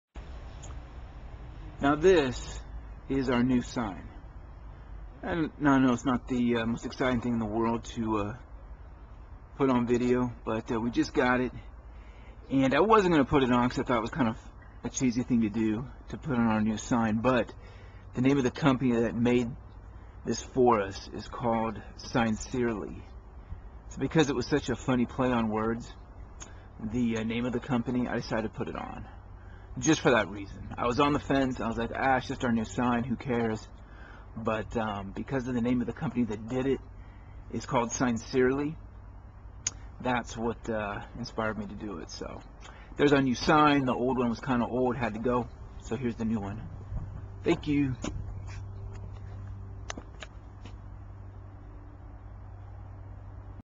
Drum Roll........The New Sign!
We got our new sign, and you have to hear the name of the company that made it.